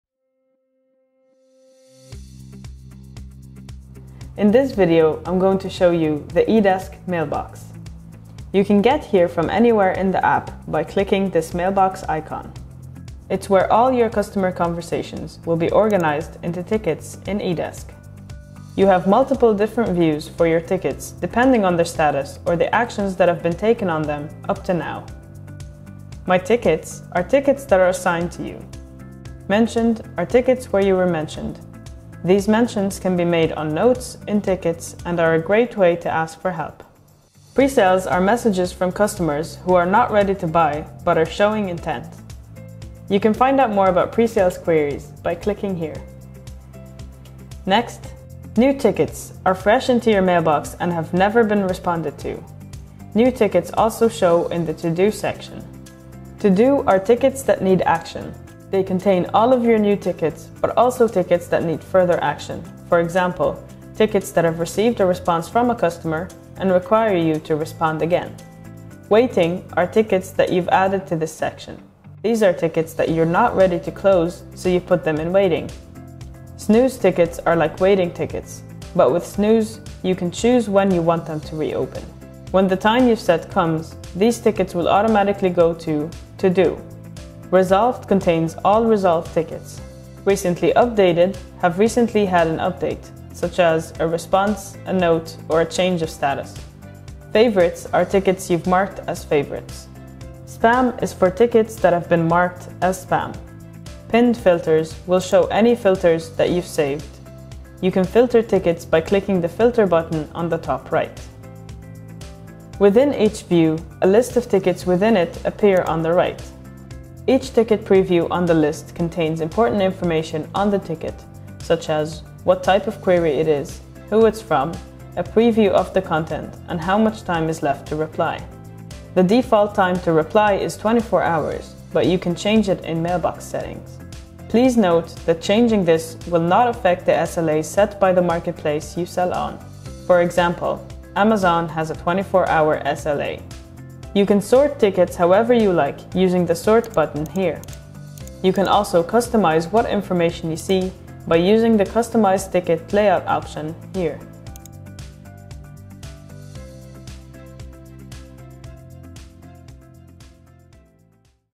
Intro to Mailbox - Overview
eDesk's powerful Mailbox gathers all your customer's interactions across all the different platforms into one single inbox.